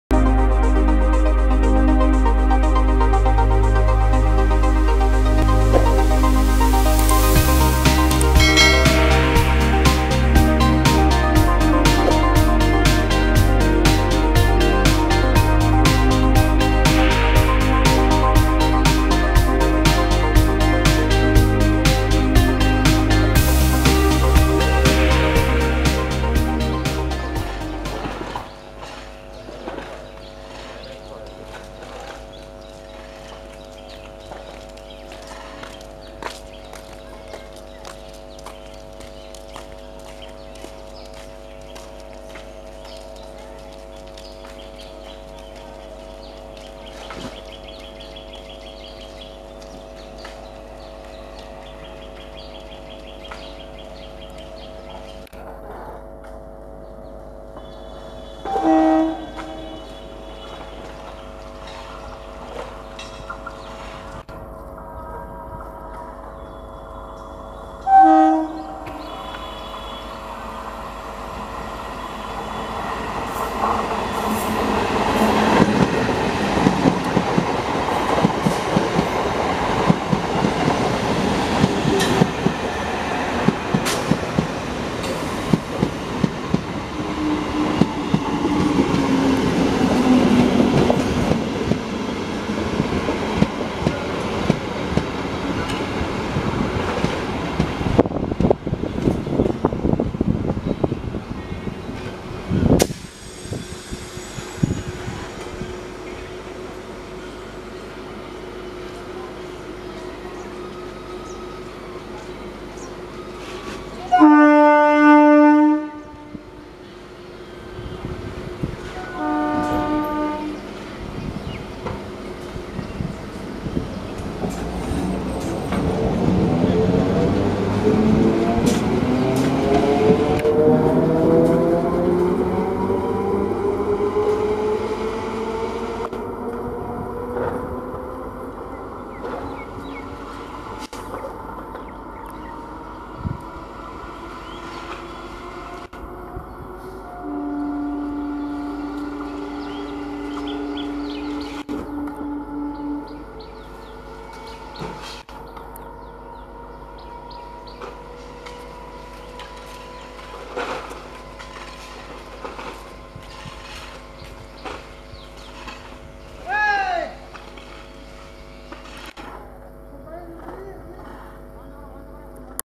Unique emu rake | The patrolling train entered the station and The staff went to their daily work
welcome
     my channel
train Galaxy,